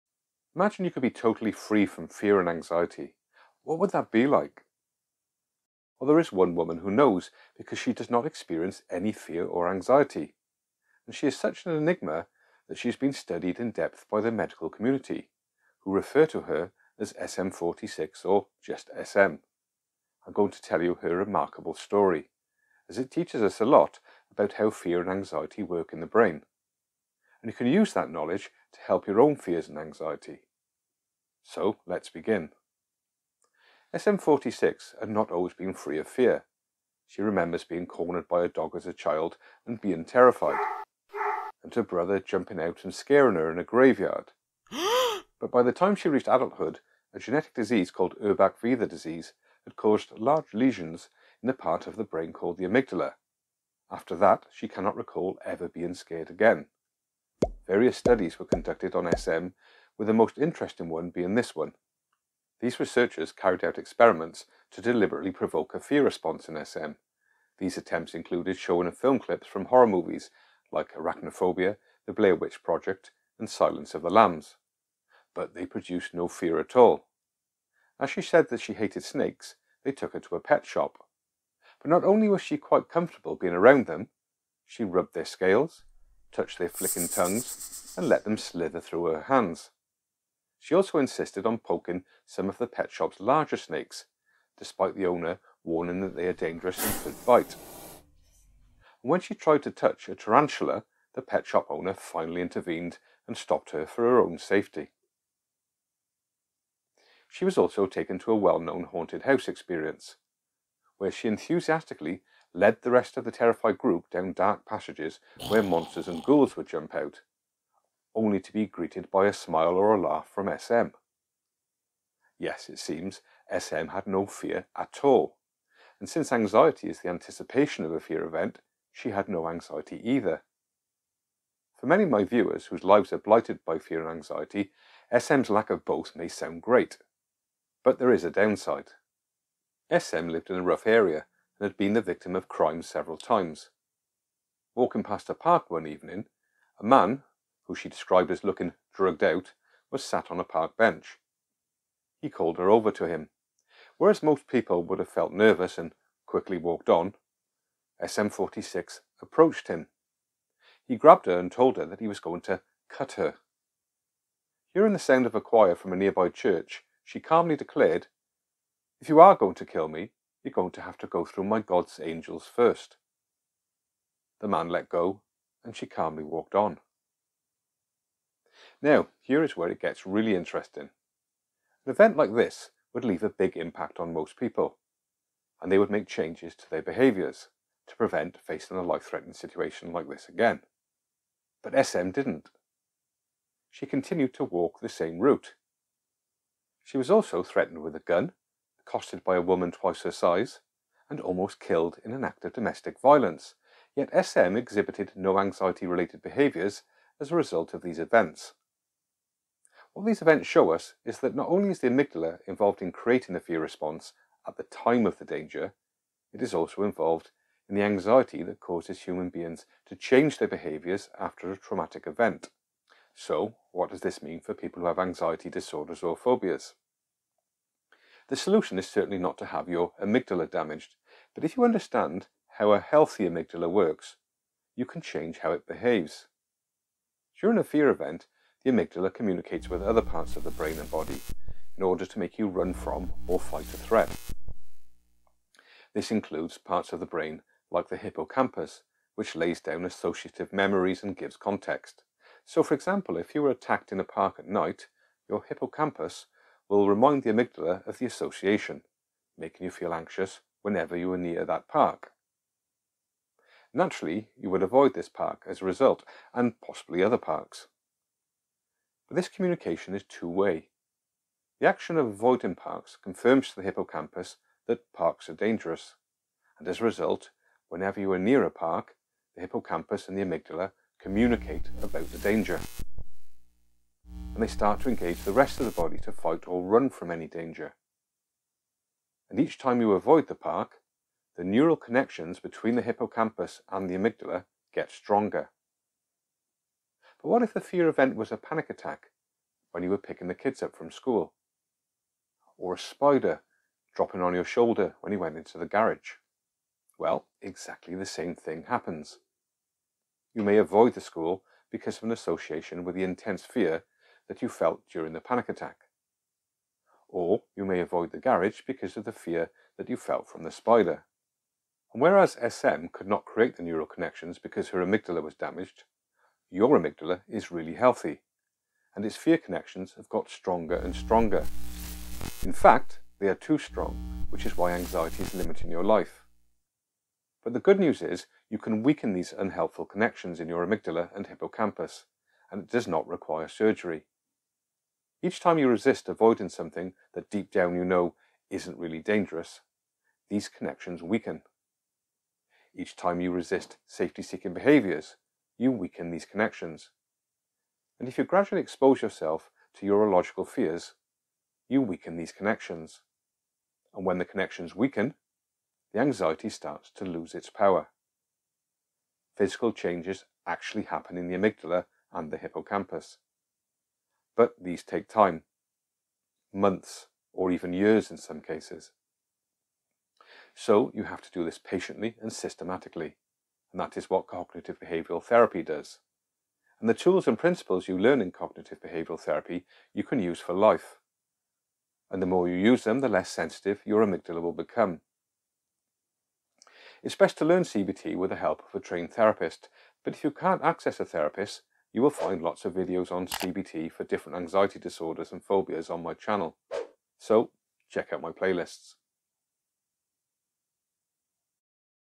How anxiety works in the brain
The woman who had no fear or anxiety taught us a lot about how anxiety works in the brain. But how did this woman known as SM046 become free of fear and what does it mean for people who have anxiety disorders like panic disorder, health anxiety, OCD and Generalised Anxiety Disorder. Does it get us any closer to a cure for anxiety? In this video we hear about a woman who's amygdala was damaged resulting in her being free of fear and free of anxiety. But is being worry free really a good thing.?